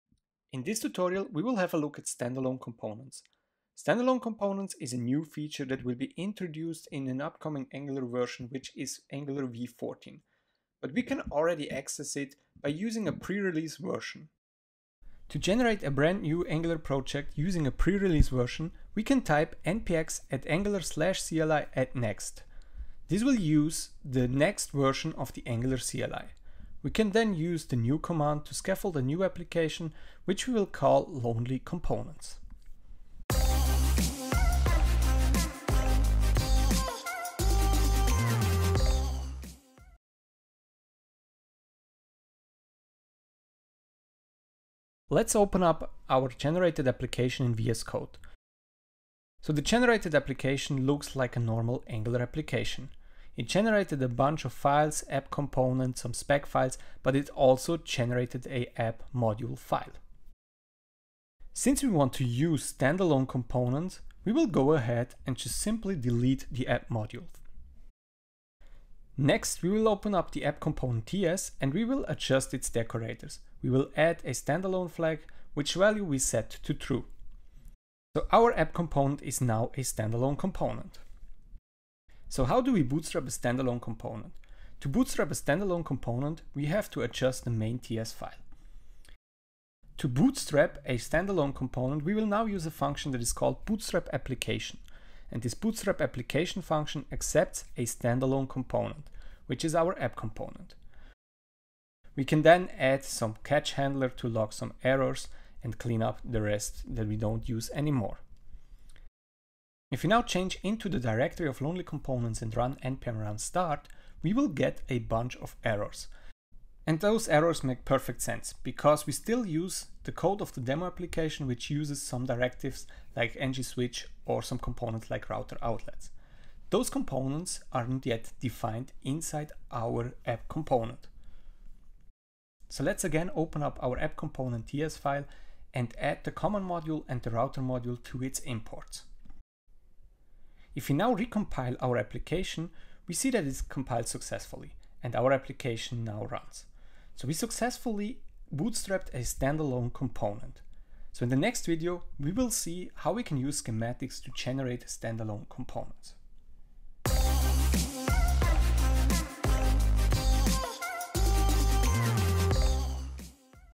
Bootstrapping standalone components in Angular 14
In this video, we will take an early look at Angular's new upcoming standalone components feature. We will learn how to create a standalone component and how to bootstrap them.

⚠️ In this video we use some prerelease API which is still experimental and might change in the final release.

Do you find the information in this video useful? — We are providing tailored expert support for developing of your Angular applications.